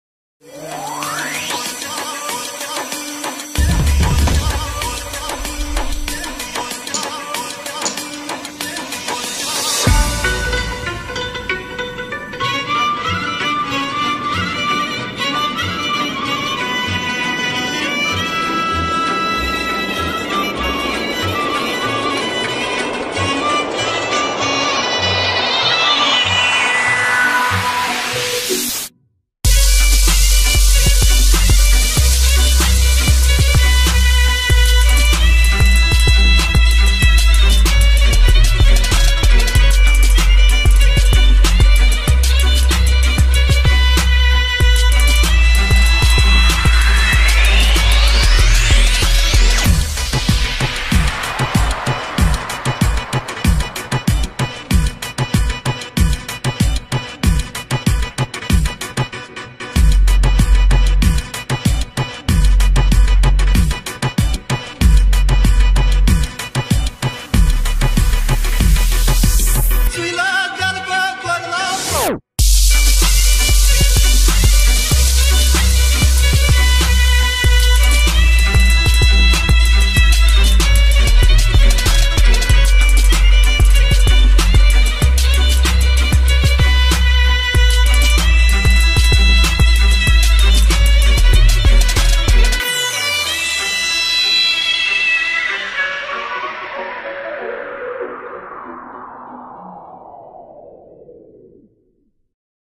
Romanian Party at Game World Bucharest Mall
The Romanian National Day was celebrated with great joy at Game World Bucharest Mall. The largest gaming hall in Romania hosted a record-breaking audience, attracted by the traditional food and drinks and the generous lottery prizes. Midnight was the moment for luck to decide the winner of the second VW Golf car awarded in the most exciting lottery: "Win a car every week at Game World!"